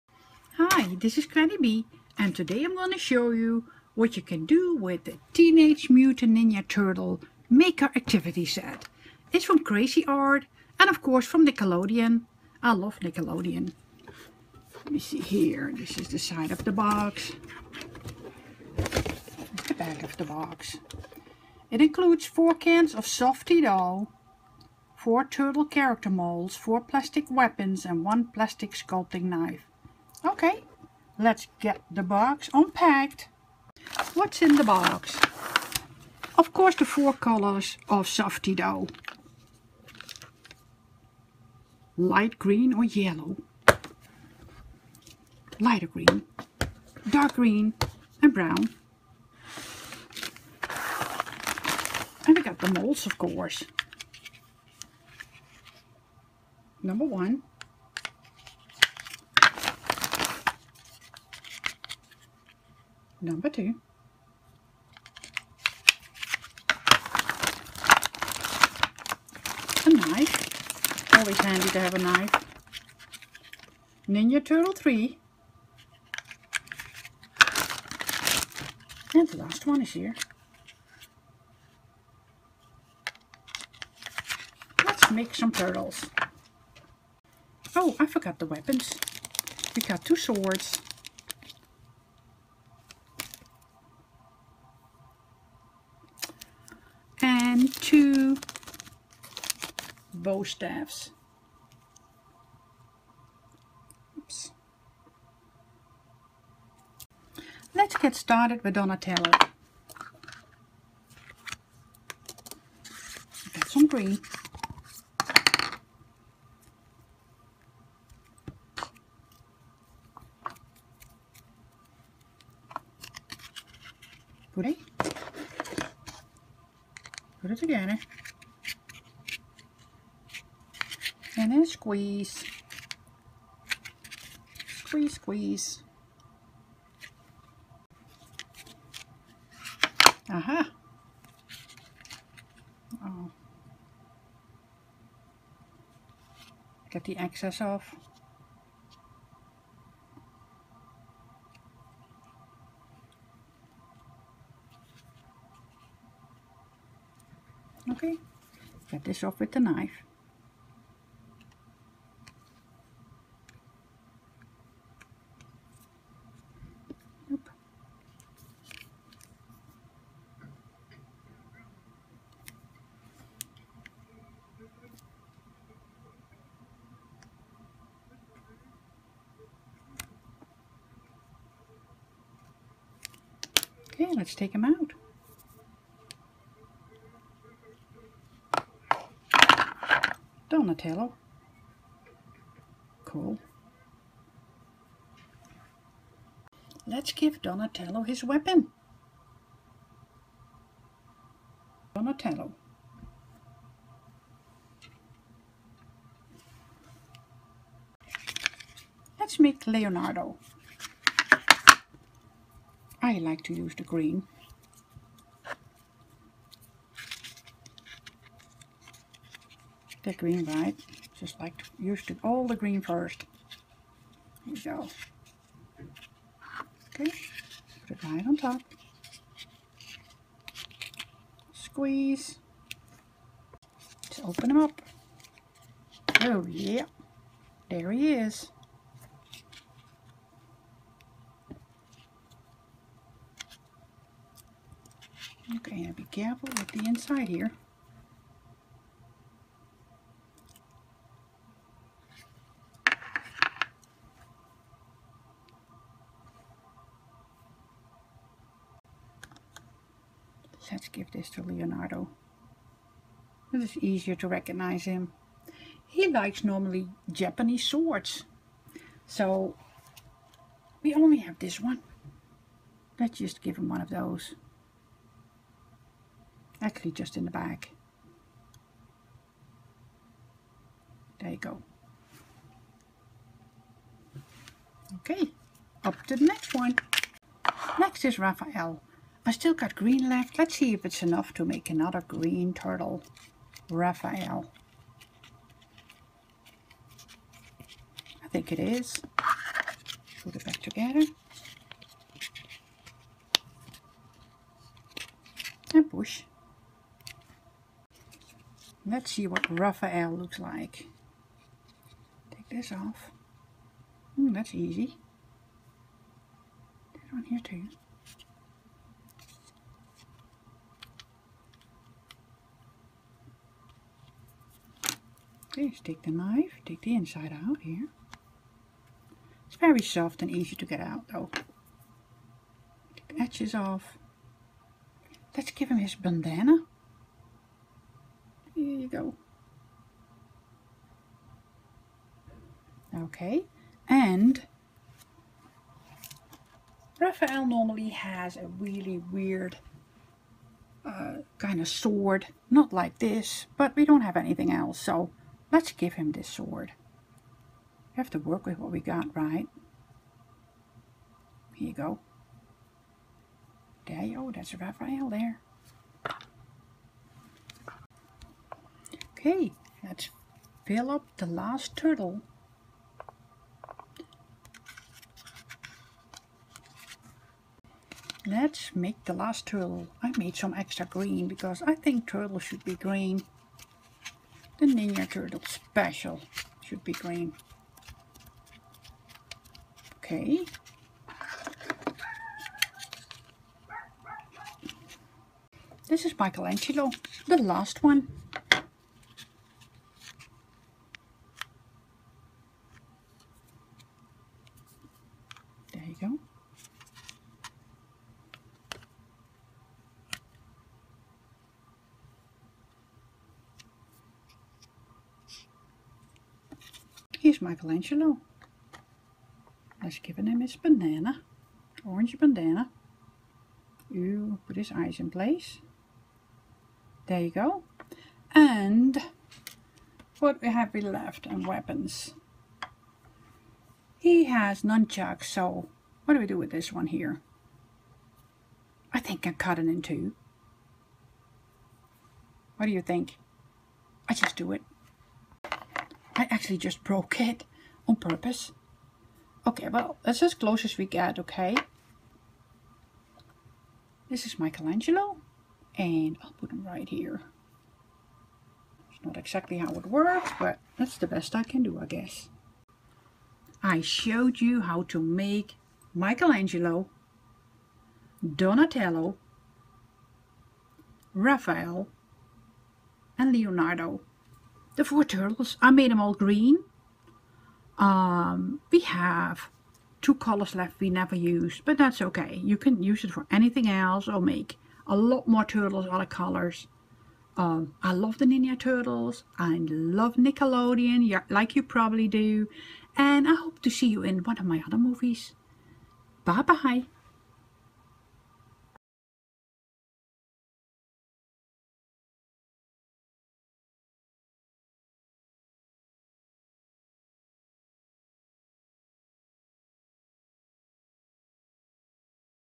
Teenage Mutant Ninja Turtles, Make your own with Cra-Z-Art
Finally you can make your own Teenage Mutant Ninja Turtles with the Cra-Z-Art Turtle Maker Activity Set.
Nickelodeon brought us The Ninja Turtles, they are so much fun.
Granny B. shows you what is in this box and how to use everything...